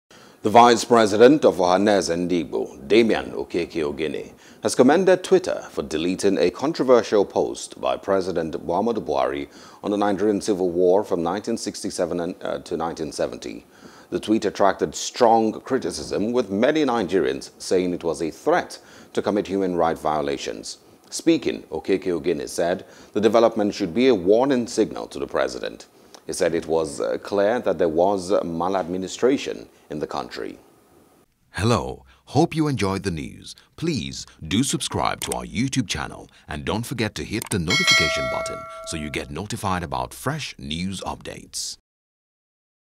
Twitter’s Action, Warning Signal To Buhari – Ohanaeze | NEWS
Vice President of Ohanaeze Ndigbo, Damian Okeke-Ogene has commended Twitter for deleting a controversial post by President Muhammadu Buhari on Nigerian Civil War from 1967 and 1970.

The tweet attracted strong criticisms with many Nigerians saying it was a threat to commit human rights violations.